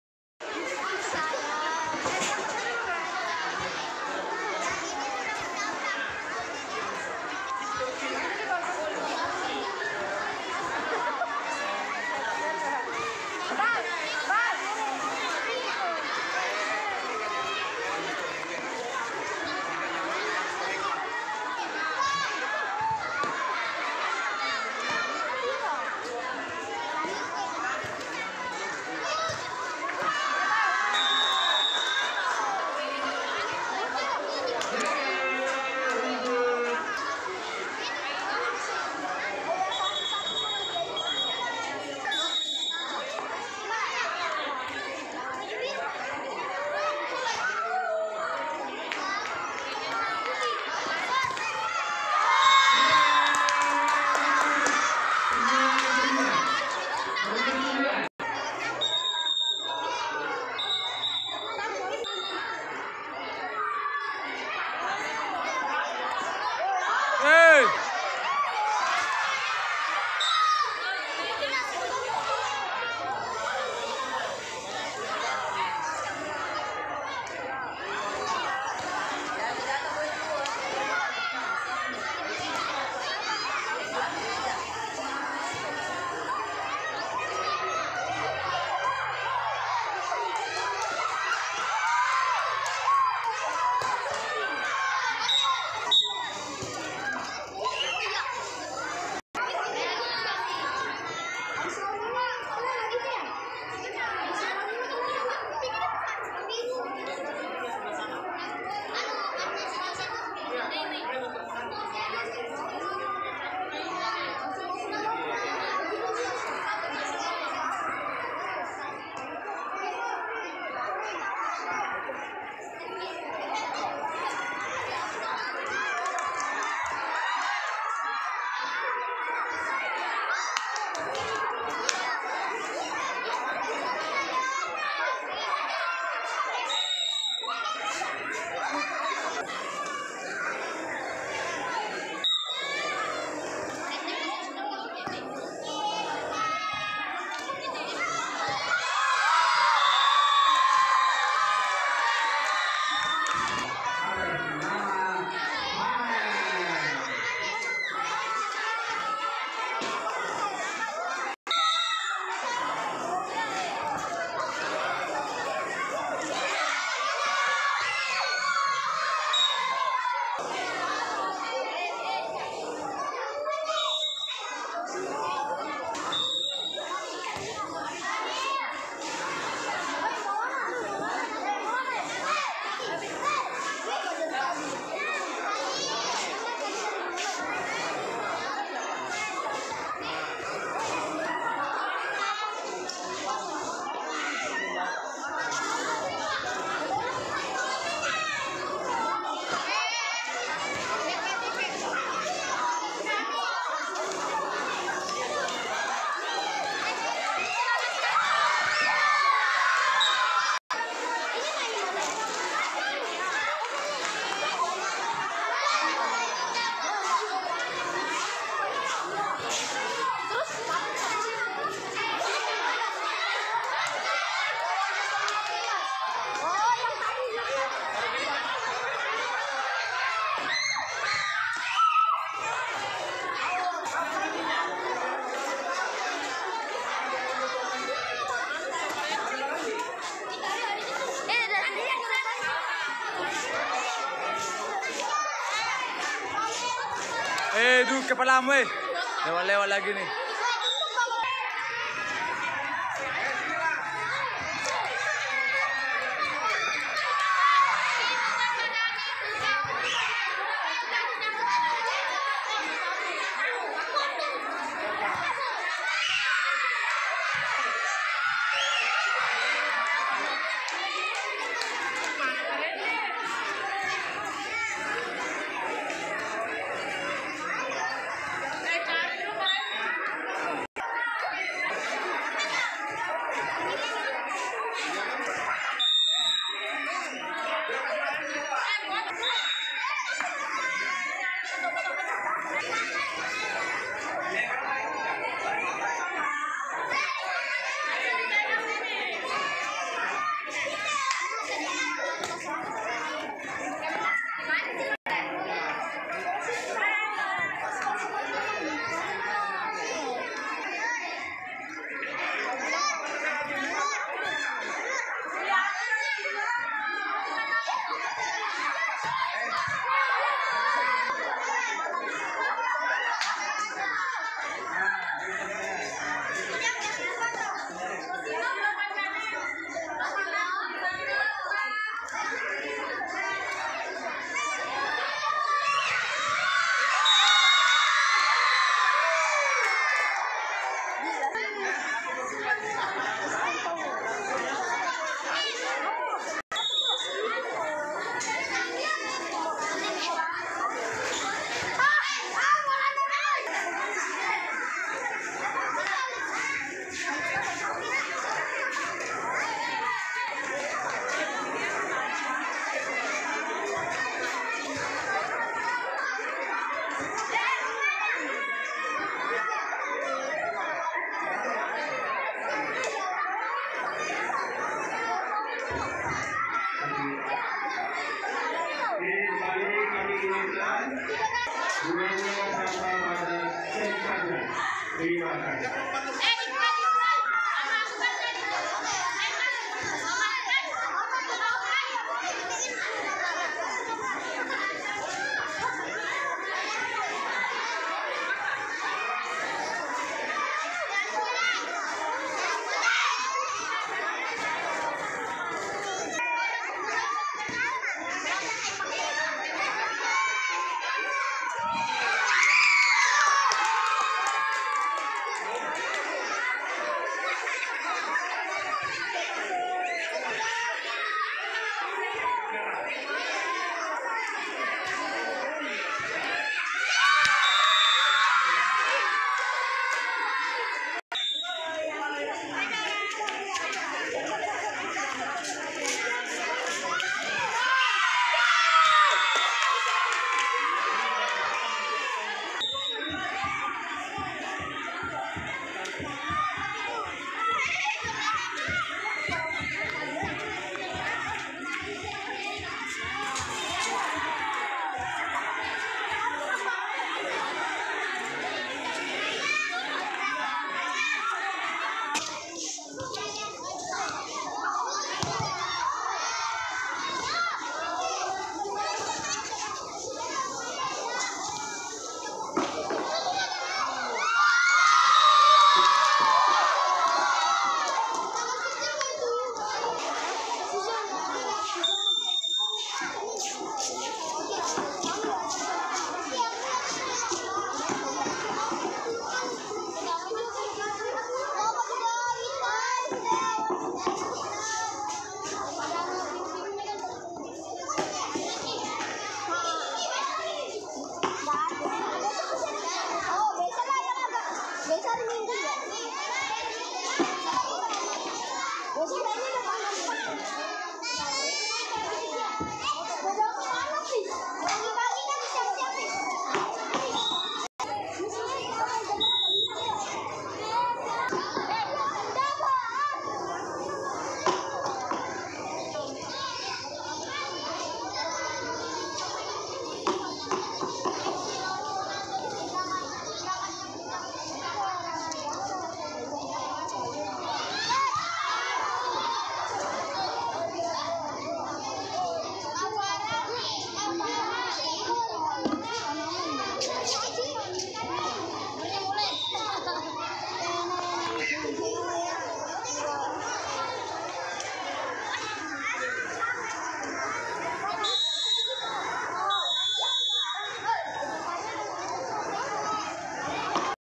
Babak Pertama Pesta Gol SDN 003 A 5 vs 0 SDN Tunas Kasih B
Tornamen KKG CUP I Kota Tarakan.
Untuk Mencari Bibit-bibit Sekolah Dasar.
Membuat Olahraga Sehat.
Salam Olahraga ✊